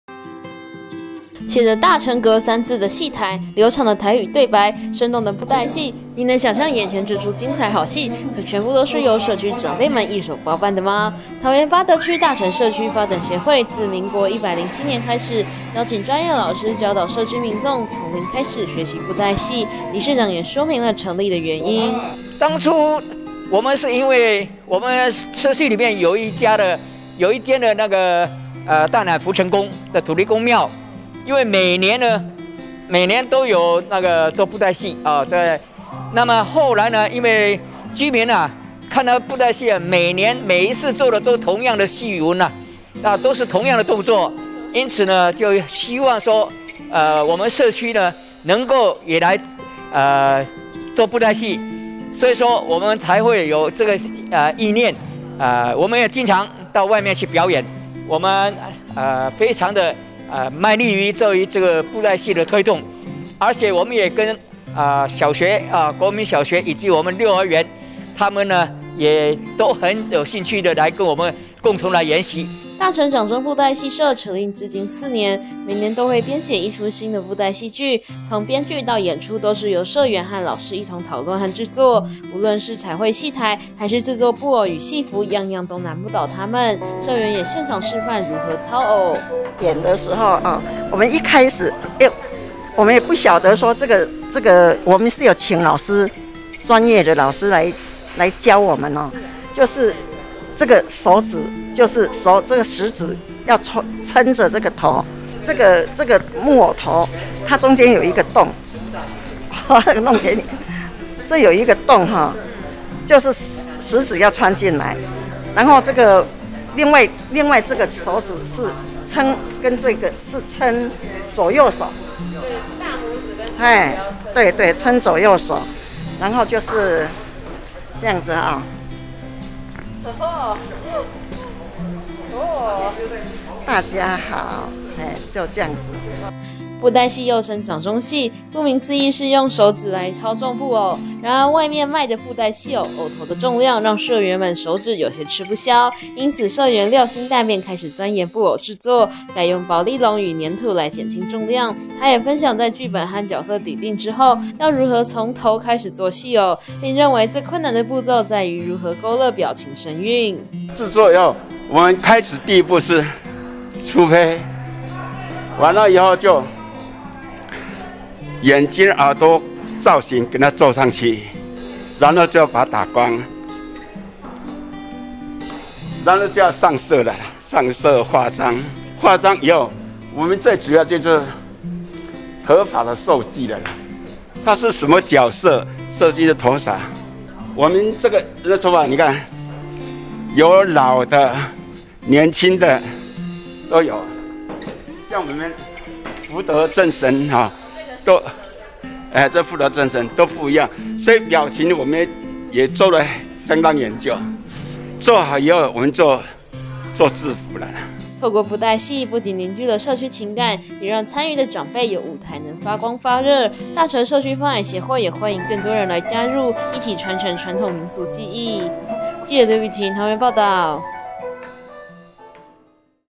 布袋戲 團|八德大成布袋戲社 從製作到演出都自己來!
八德大成社區掌中布袋戲社團團員們從零開始學習到現在能一手包辦製作到演出

[中嘉新聞/北健/桃園八德報導]
桃園八德區大成社區發展協會自民國107年開始，邀請專業老師來教導社區民眾，從零開始學布袋戲，如今從彩繪戲台到製作布偶與戲服，從編劇到搭配音樂演出，學員們樣樣都能自己來，一手包辦搞定每次表演，不僅能傳承傳統民俗技藝，也讓參與的長輩有舞台能發光發熱。